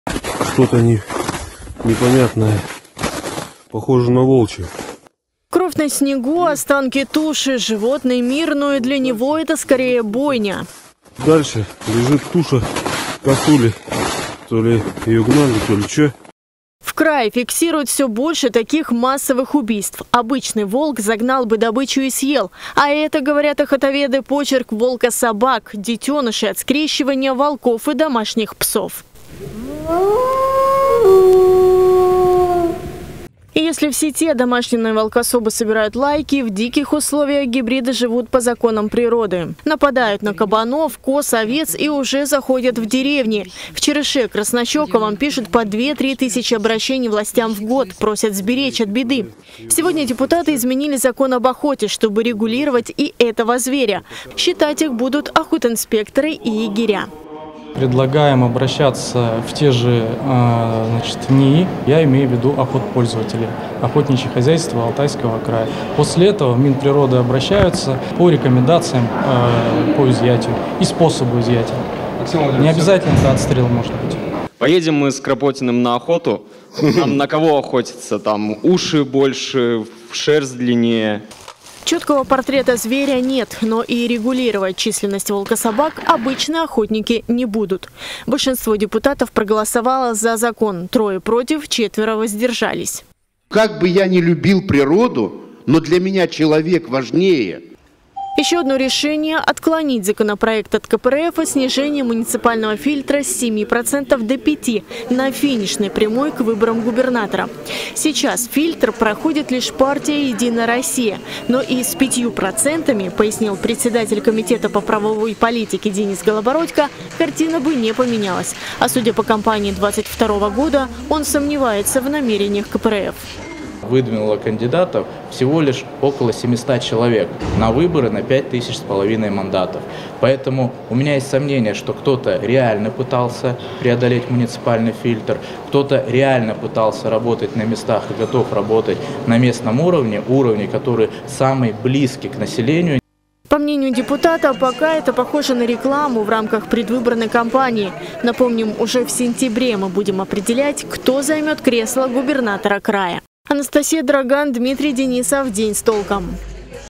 Алтайские депутаты разрешили отстреливать волкособак в охотугодьях
Пока чуть ли не каждую неделю в крае на людей нападают бродячие собаки, появилась новая угроза – волкособаки. Это гибрид волка и собаки, который распространен в лесной местности. Об их опасности заявили краевые парламентарии – и скорректировали сегодня закон об охоте. Готовиться ли к массовому отстрелу?